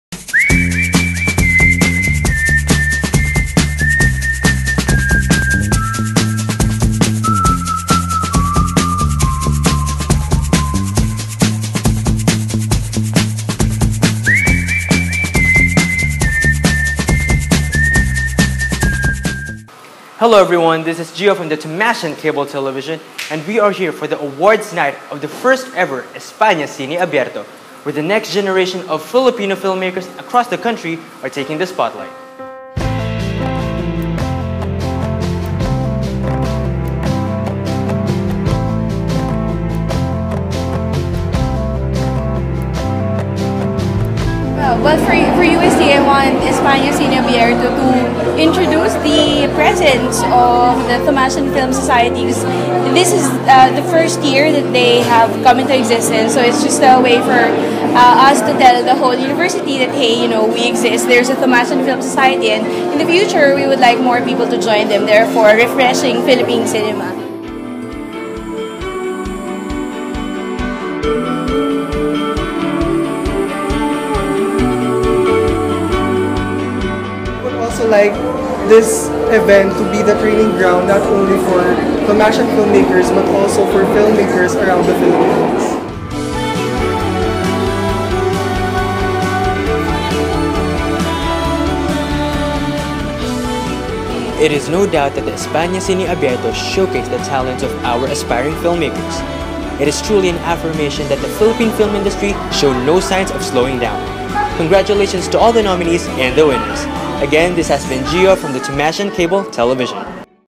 TommyGoesTo España Cine Abierto
Let us check in with Gio as he accompanied Tommy to the Awards Night of the first ever España Cine Abierto!

Executive Producer: Phileo Victor Ko
Videographer/Editor: Hannah Dela Cruz
Talent: Gio Gamban